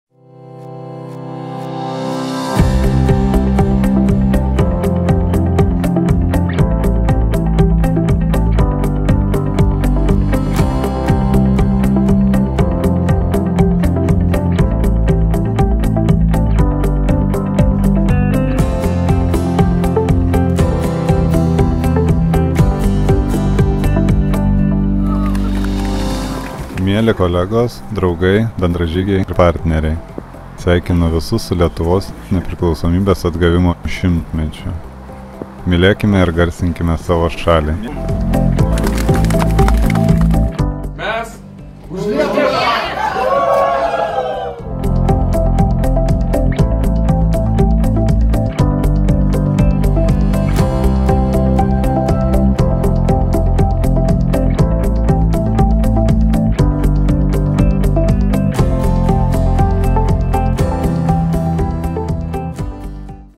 Happy 100th Anniversary of restored Independence of Lithuania!
This year, February 16 is extraordinary for Lithuania. One hundred years have passed since the restoration of Independence of Lithuania! Avia Solutions Group congratulates all people of Lithuania on this significant day and encourages everyone to celebrate our freedom.